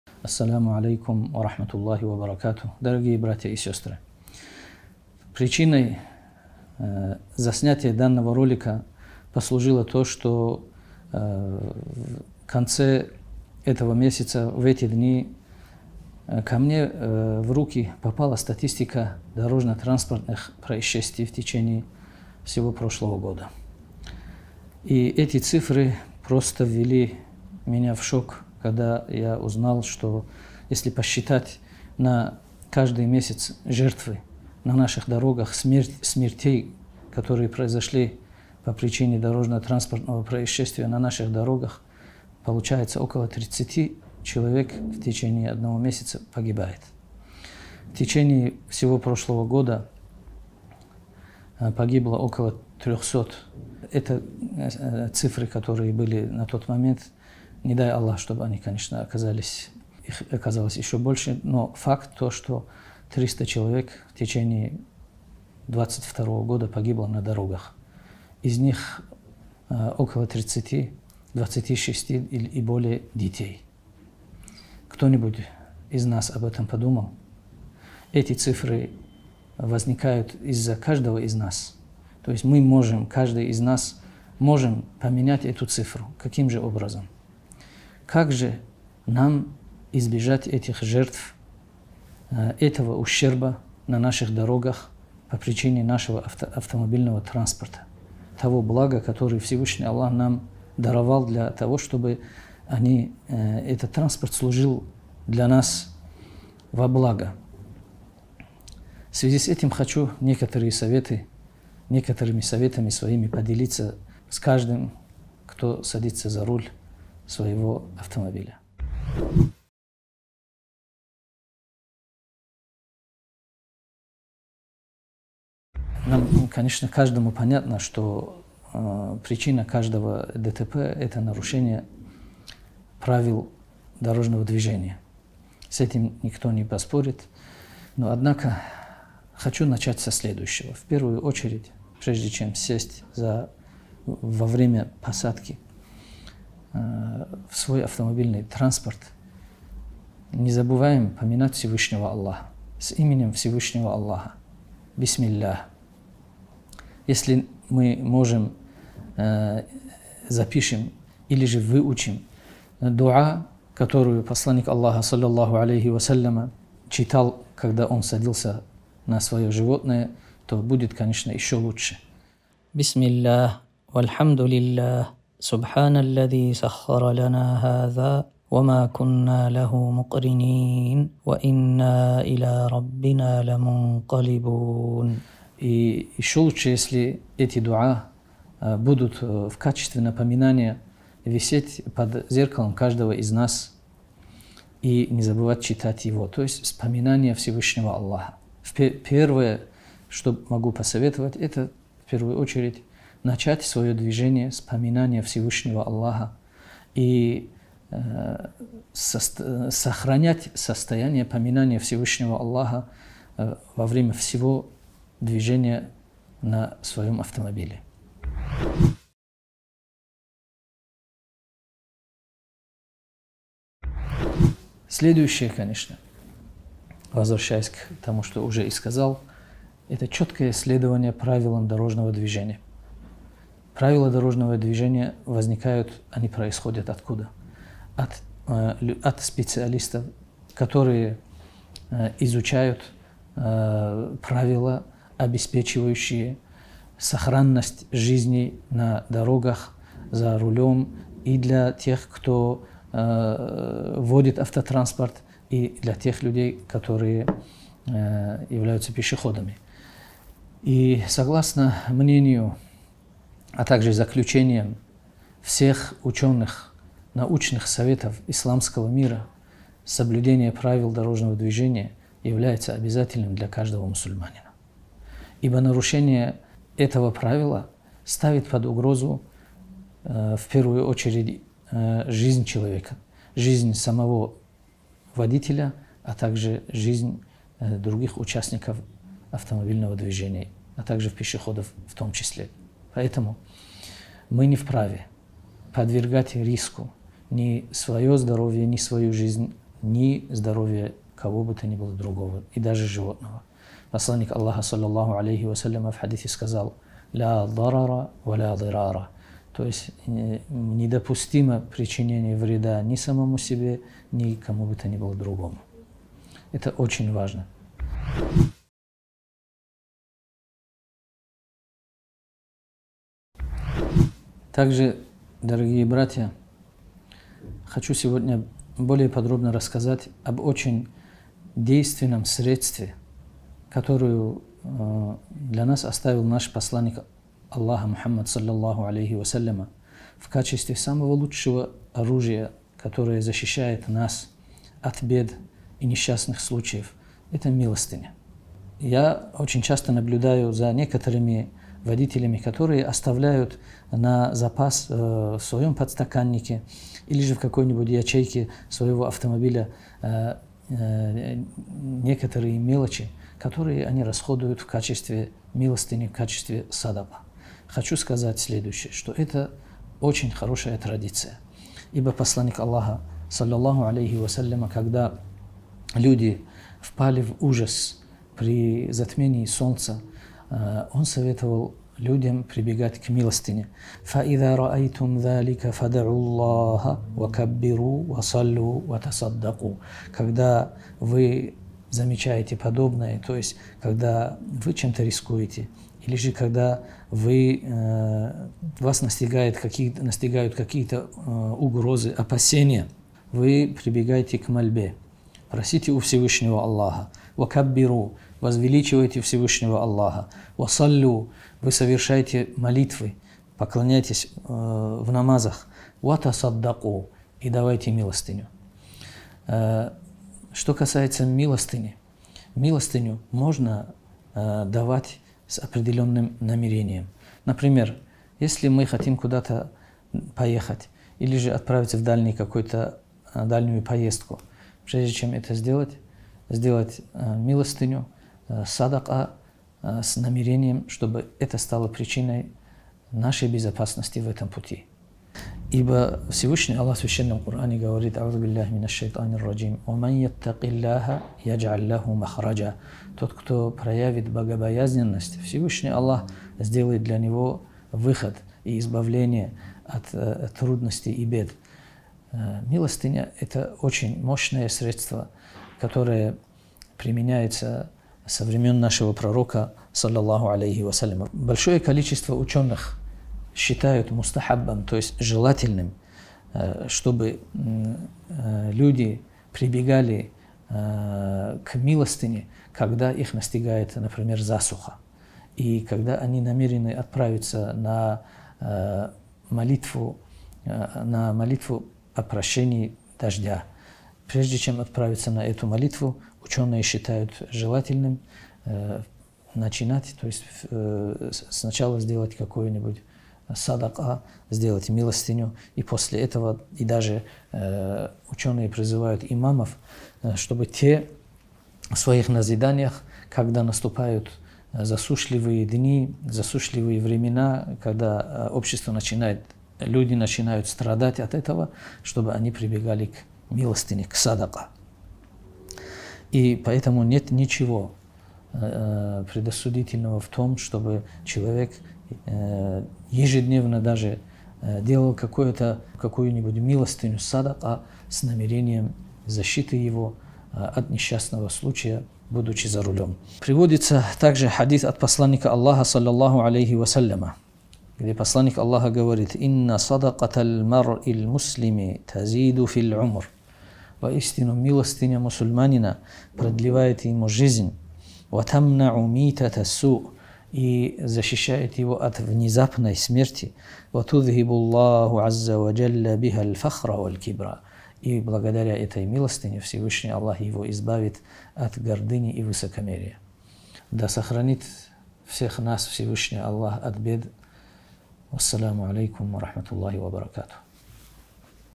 Дагестан. 300 смертей в 2022 году. / Саадуев М-Расул
Всевышний Аллах через своего Посланника Мухаммада (мир ему и благословение Аллаха) передал людям знания обо всём необходимом. Пророк рассказал нам, каким должно быть поведение человека во всех сферах жизни.

Но, несмотря на всё это, в последнее время то ли из-за стремительного роста числа автотранспорта, то ли из-за водителей, которые очень халатно относятся к этике вождения и правилам дорожного движения, происходит очень много аварий.

Каждый водитель должен знать, что из-за его халатности и безответственности, из-за несоблюдения правил обслуживания и вождения транспорта может произойти ДТП. Также он должен всегда помнить, что ему придётся держать ответ за это и на этом свете, и в потустороннем мире.

Немаловажную роль в предотвращении ДТП и защите водителя и пассажиров от неприятностей в пути играет и то, как водитель готовился к поездке. Если он начал свой путь с поминания Всевышнего Аллаха, обращаясь к Нему с мольбами, в дороге ему непременно будет сопутствовать баракат.